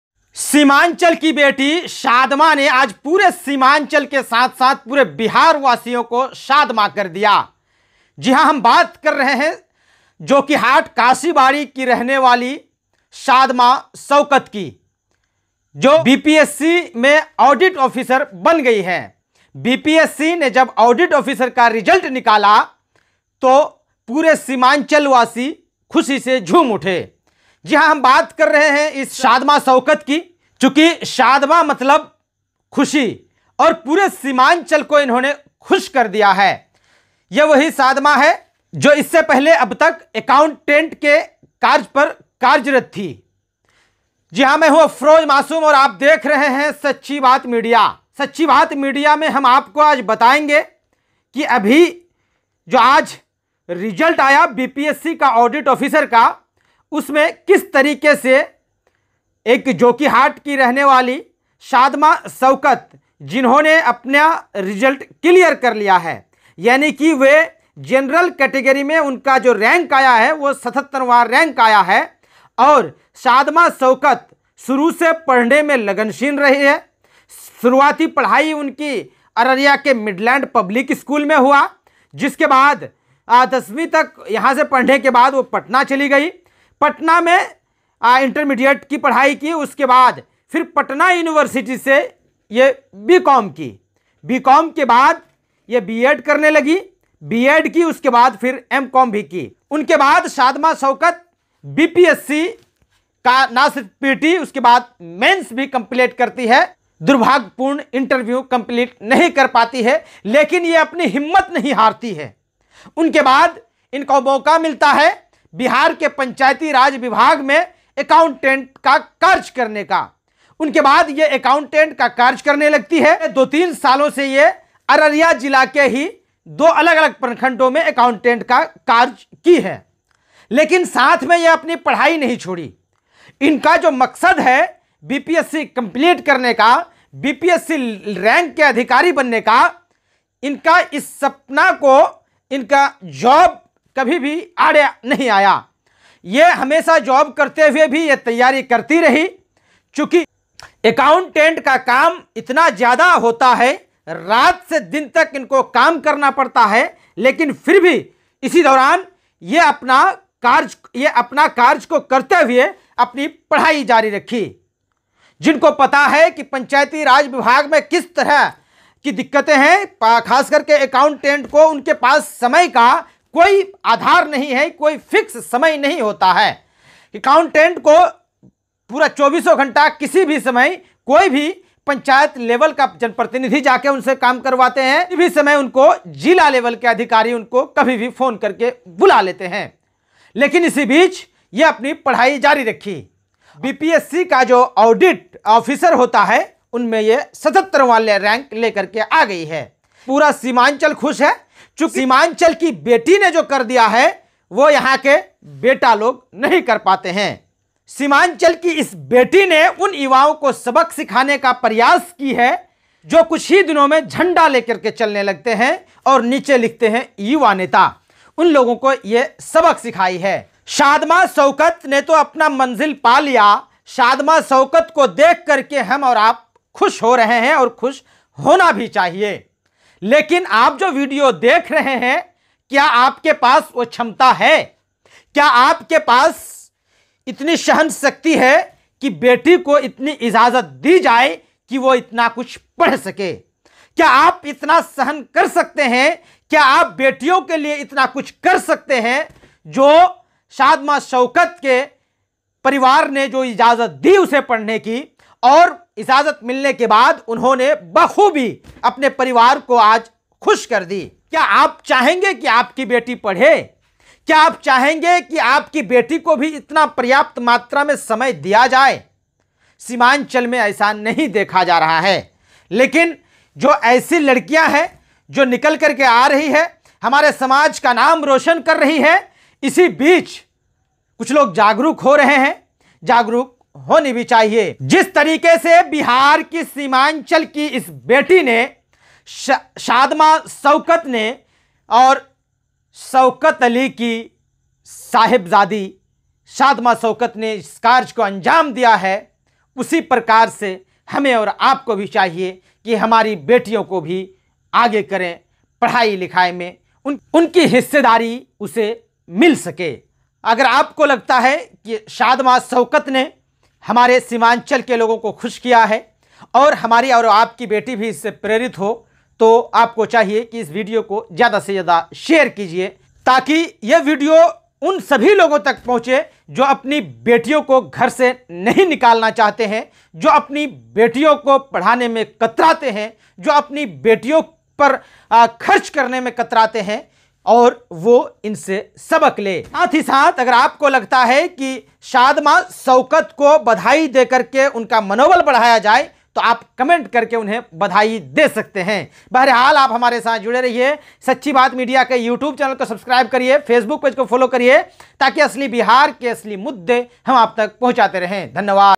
सीमांचल की बेटी Shadma Shaukat ने लहराया परचम। BPSC पास कर बनी ऑडिट ऑफिसर। Jokihat की बेटी है शादमा
Shadma Shaukat जोकीहाट के काशीबाड़ी की रहने वाली है। इससे पहले BPSC Exam में Mains भी पूरी की थी।